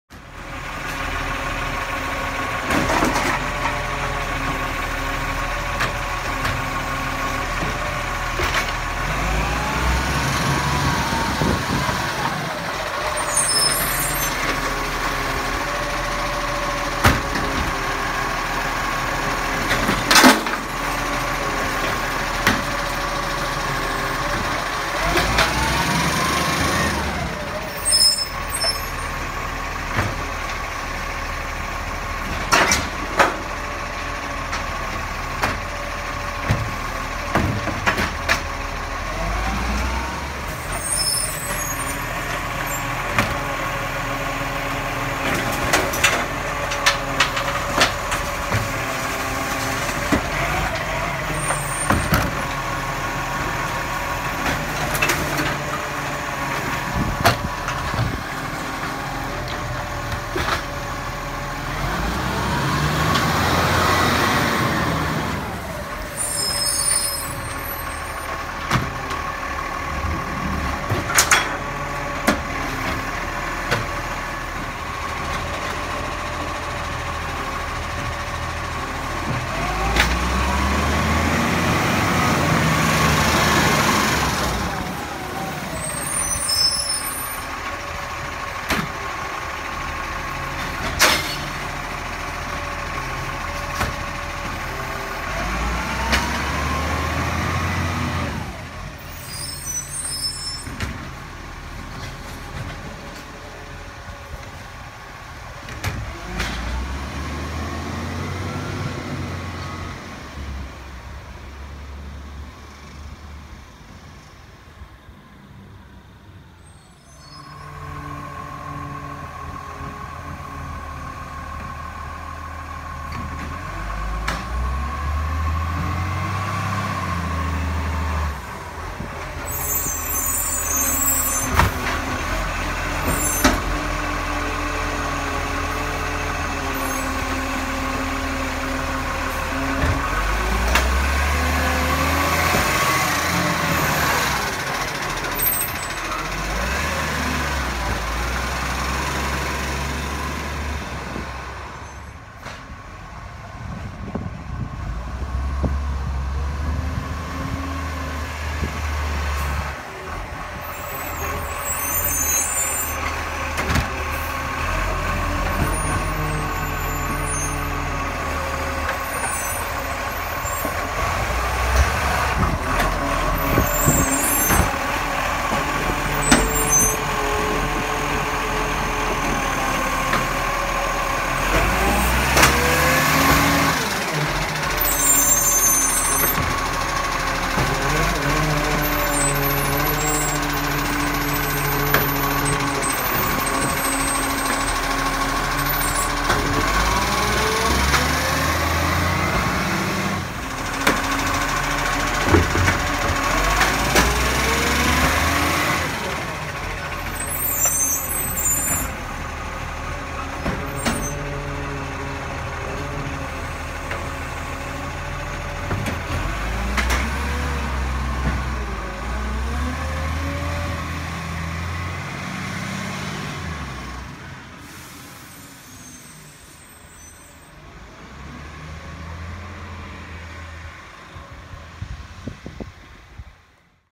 Glen Eira Recycling And Green Waste
Merry Christmas!!

Here we have two Glen Eira Trucks Collecting In Bentleigh East on A Monday witch was Christmas Day. Hope u enjoy have a great awesome and safe Christmas - Paz